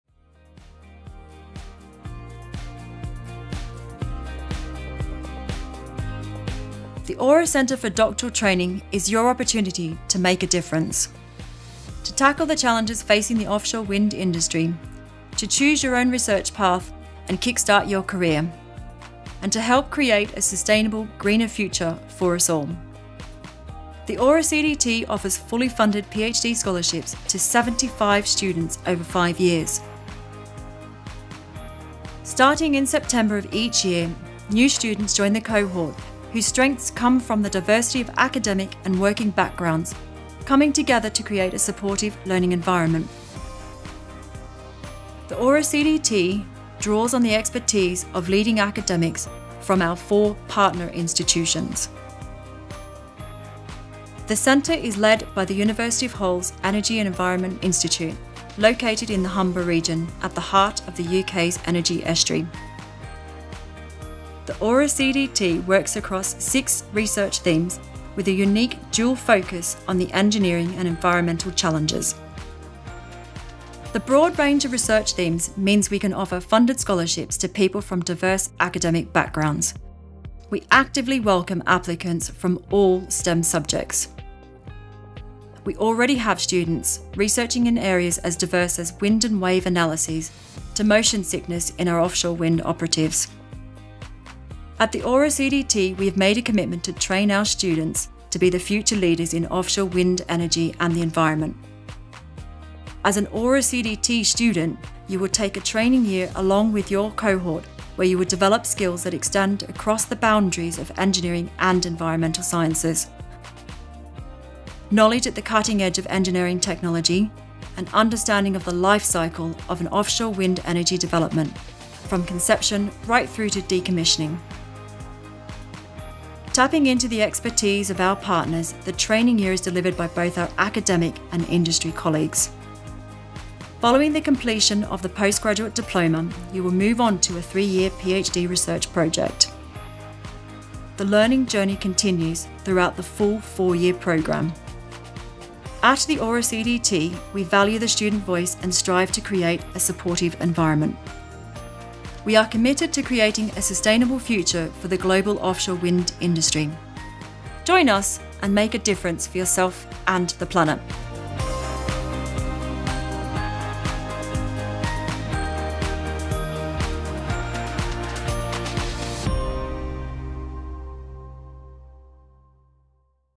Funded PhD Scholarships in Offshore Wind Energy and the Environment at the Aura CDT
Find out more about funded PhD Scholarships at the EPSRC-NERC funded Aura Centre for Doctoral Training (CDT) in Offshore Wind Energy and the Environment. We take graduates from all STEM subjects to research a wide range of challenges facing the offshore wind industry to support the sustainable growth of the sector and contribute to society's transition to a zero carbon future.
The Aura CDT is a partnership between the Universities of Hull, Durham, Newcastle and Sheffield and brings together each institutions core strengths in environmental science and engineering. The Aura CDT's core research themes have been developed in conjunction with industry, to ensure that the research is relevant to the challenges facing the offshore wind sector; we also offer a number of co-funded scholarships in partnership with key employers.